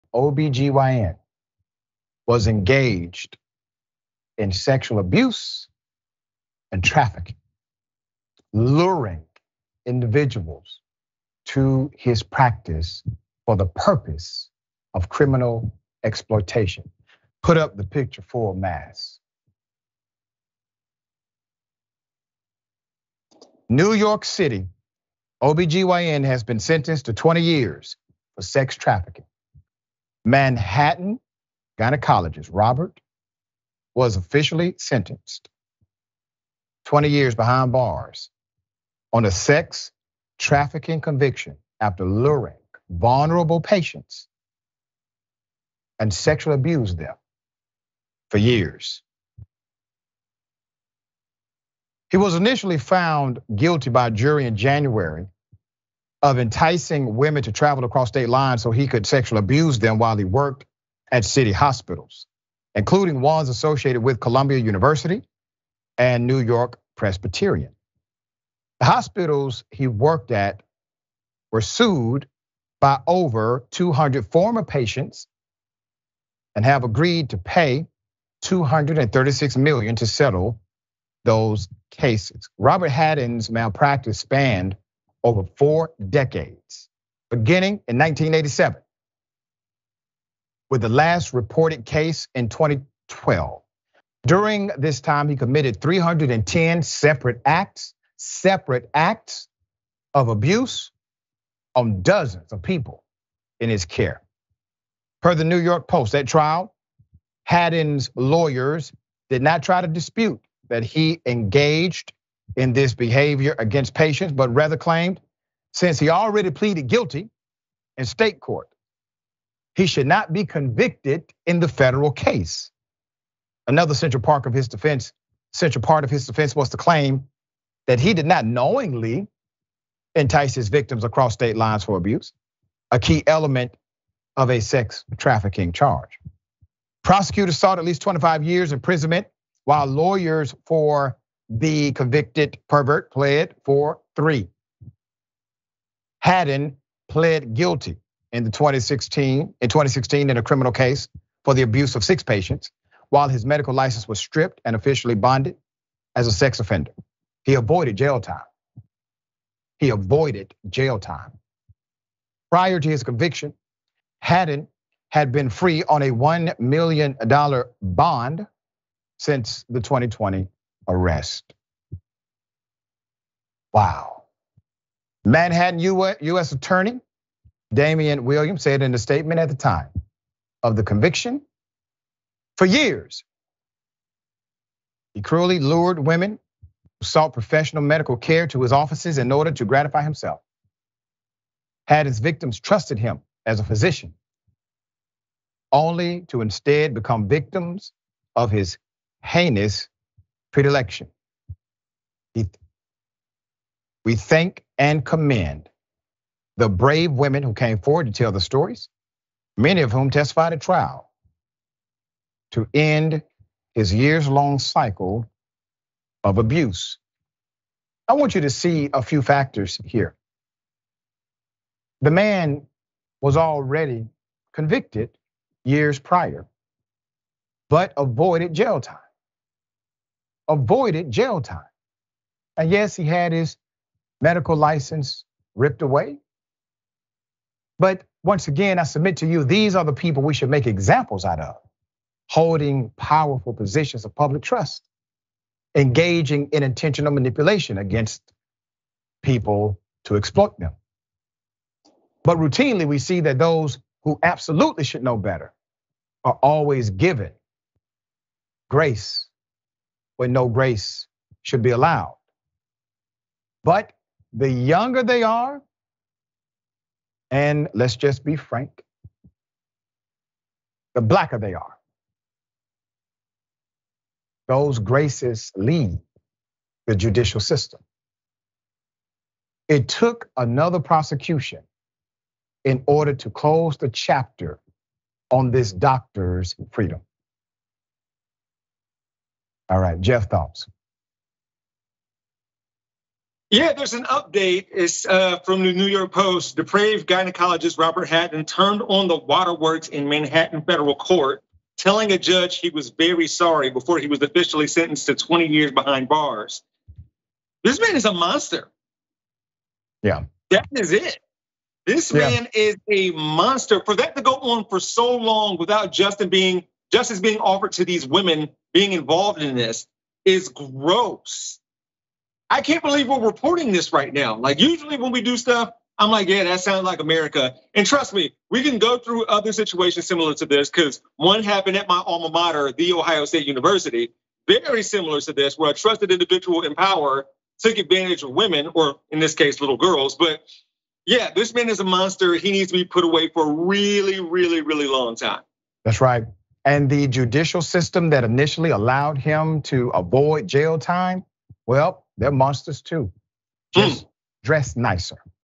Disgraced Gynecologist Faces 20-Year Sentence For Sex-trafficking
New York City physician, monster Robert Hadden has been sentenced to 20 years in prison for sex trafficking and sexually abusing hundreds of his patients. Hadden was a sexual predator disguised in a white coat. Dr. Rashad Richey and Jeff Wiggins discuss on Indisputable.

Disgraced Manhattan gynecologist Robert Hadden is set to be sentenced to 20 years behind bars on a sex-trafficking conviction for luring vulnerable patients so he could sexually abuse them, a federal judge said Monday.

Manhattan federal Judge Richard Berman said he planned to hand down the punishment Tuesday after hearing emotional statements from 11 of Hadden’s victims last month — who described the toll the abuse had on their lives and asked that the depraved doctor be locked up for as long as possible.

“Robert Hadden is a sexual predator disguised in a white coat,” said one woman, who used the pseudonym Emily Anderson.

Hadden, 64, of Englewood, New Jersey, was found guilty by a jury in January of all four counts of enticing women to travel across state lines so he could abuse them while he worked at prestigious city hospitals, including ones associated with Columbia University and New York-Presbyterian.

Prosecutors sought at least 25 years imprisonment, while lawyers for the convicted pervert — who has been jailed for the last several months — asked that he be given just a 3-year prison term.

He had faced up to 80 years in prison at his sentencing.

230726__ID08NYCOBGYN